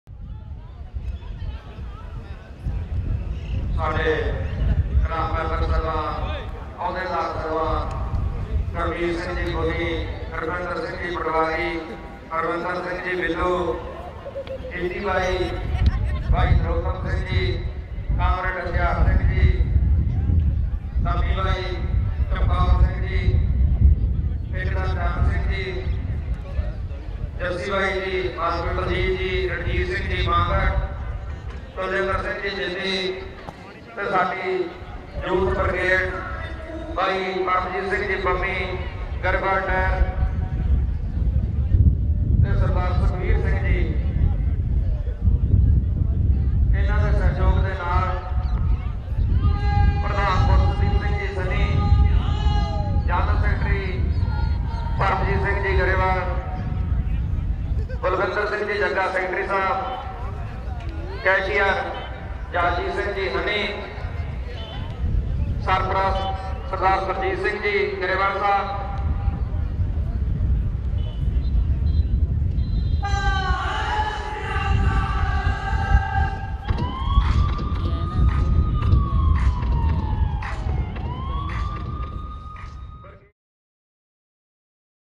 Punjabi boys ready to start the game of hockey at Rural Olympics
Hockey players in full action during the hockey competition at the Rural Olympics, Kila Raipur - Ludhiana.

Kila Raipur Sports Festival, popularly known as the Rural Olympics, is held annually in Kila Raipur (near Ludhiana), in Punjab, India.

Competition is held for major Punjabi rural sports, include cart-race, rope pulling and many more, rather strange expressions of strength like holding bicyle with ones teeth, bull lock chariot riding, pulling vehicles with hair and letting a tractor pass over your ribcage.

In February each year, Ludhiana becomes the destination for hundreds of sports enthusiasts, including foreigners. They come to Kila Raipur to see the special breed of bullocks, camels, dogs, mules and other animals competing in competitive events. The most prestigious winners have attended. In the year of the Olympics 2008, Hakam and Naib Singh Dhaliwal from village Kalsian, Punjab took the 1st place prize. They also won first in Gujjaral and Phalewal. They are known in Punjab as the men who hold the greatest passion in the sport.

Source: Wikipedia